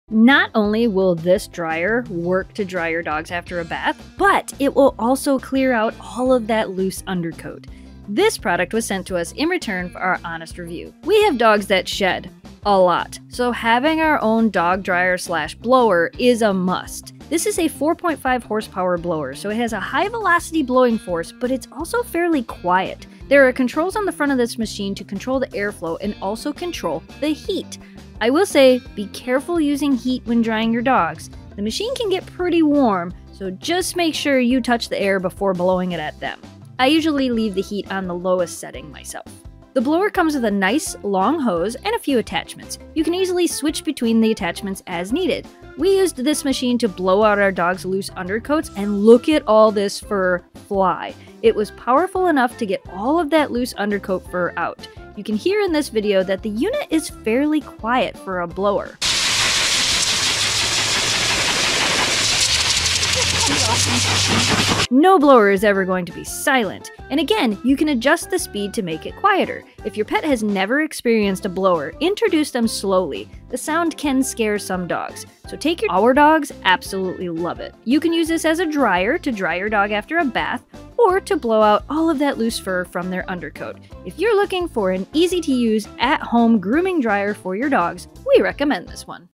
Professional Results at Home With THIS Dog Blower Dryer!
Is this the Best Dog Blow Dryer you can buy?  Let's find out!
Not only will this work to dry your dogs after a bath, but it will also clear out that loose undercoat!  This product was sent to us in return for our honest review.  We have dogs that shed a lot, so having our own dog dryer is a must!

This is a 4.5 horsepower blower, so it’s has a high velocity blowing force, but it’s also fairly quiet!  There are controls on the front of the machine to control the air flow, and also to control the heat!  I will say, be careful using heat when drying your dogs, this machine can get pretty warm.  I usually leave the heat on the lowest setting.

This blower comes with a nice long hose, and a few attachments. You can easily switch between attachments as needed. We used this machine to blow out our dogs lose undercoats, and look at that fur fly!  It was more than powerful enough to get all that loose fur out!  You can hear in this video, that the unit is fairly quiet for a blower.  No blower will ever be silent, and again you can easily adjust the speed to make quieter.  If your pet has never experienced a blower, introduce them slowly, as the sound can scare some dogs.  Take your time, and eventually our dog will learn to enjoy this process!  Our dogs LOVE it!

Again you can use this as a dryer, or to blow out lose fur from their coat.  If you are looking for an at home grooming drying for your dogs, we recommend this.